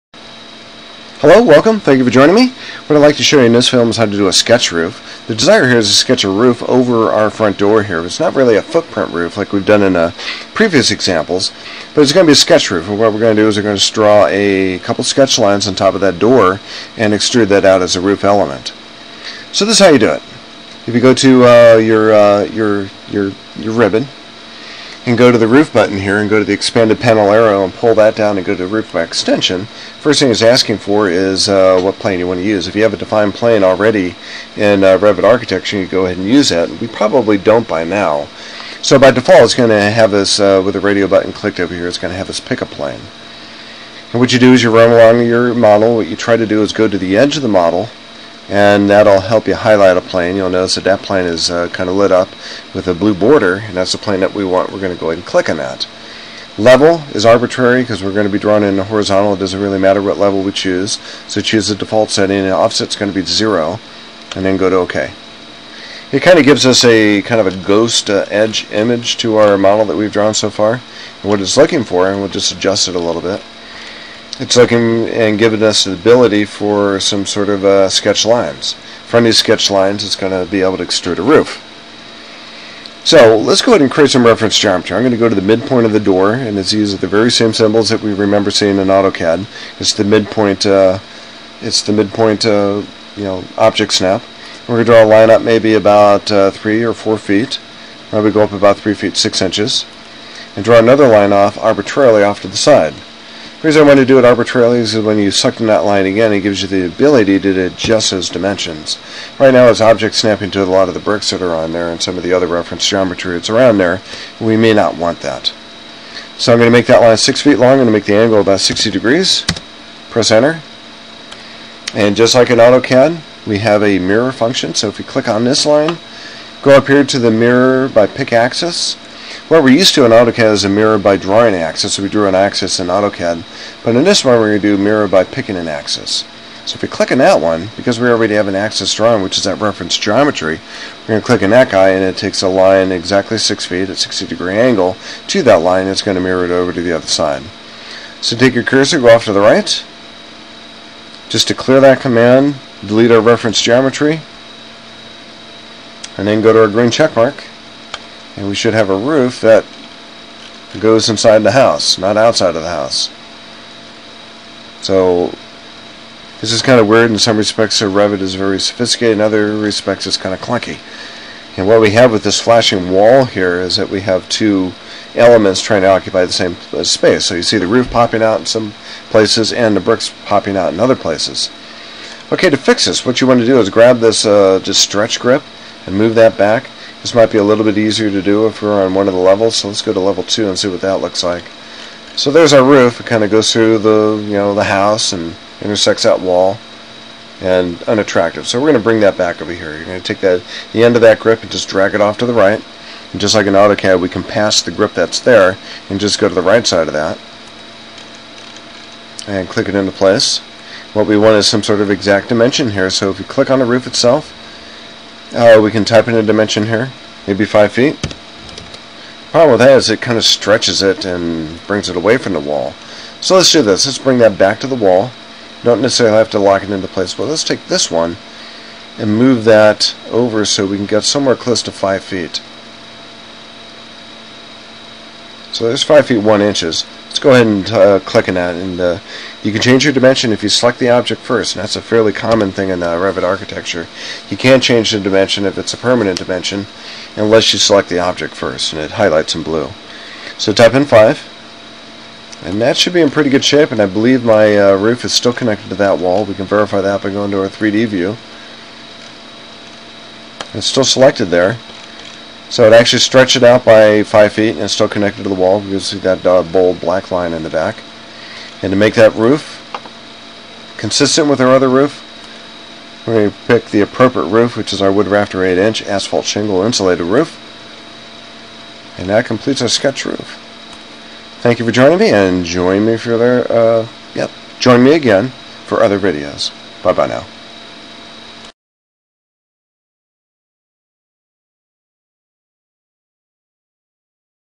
Revit, Project Basics – Create a Roof 5 – Roof by Extrusion, Over Door, Sketch lines, Grips Pull Out
Revit Architecture Week 4 Assignments, Roof by Extrusion – With Sketch Lines, Roof Over a Door, Pick plane with Edge of Face, Sketch lines with Reference Geometry, Use Grips on Plan View to Pull Roof Outside

Let’s look at a sketch roof, a roof covering over a door a floor down from our 2 story house.  Go to the roof button on the Ribbon, expand arrow – choose Roof by Extrusion, choose plane in the modeling area – pick a plane radio button, pick the edge of a face (to select the plane you are looking for (tab key otherwise)), face will light up as a ghost edged image showing that it is selected, front on view and sketch some lines.  Reference geometry first - midpoint object snap like in AutoCAD, sketch line up 3’ 6-00” (3 and a half feet), from the top of that line sketch and line to the right at an angle, click on that line again to adjust the linear and angular dimensions (6 feet and 60 degrees), mirror sketch option – mirror by axis – line on the right for a line on the left, notice magenta lines for our geometry that we can use, now we have the lines for our Roof by Extrusion.  Click on Roof by Extrusion button, flip the direction (it is not a roof extending to the inside of the house - yikes), flashing wall are 2 planes occupying the same space, grab the end grip from the Level 2 Floor plan and extend it out to the outside on top of the door, select it again to take the Temporary Dimensions and convert them into Permanent Dimensions with specific values, lock the padlock on existing wall.  Make consistent with the exiting roof on the house.